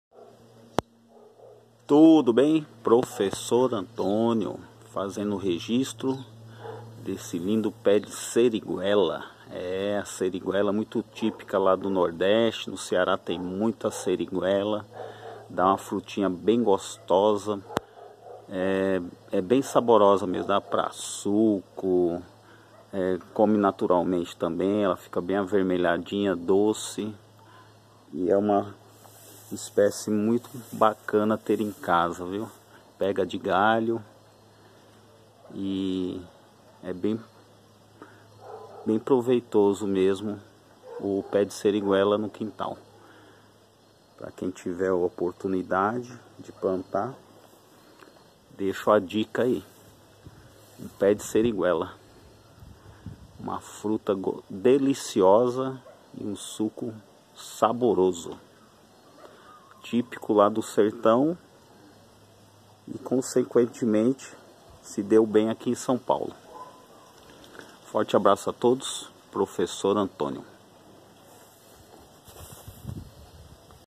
Seriguela! Típica do Nordeste. Cidade Tiradentes, Sp
Registro frutas típicas do nordeste que se adaptar amém São Paulo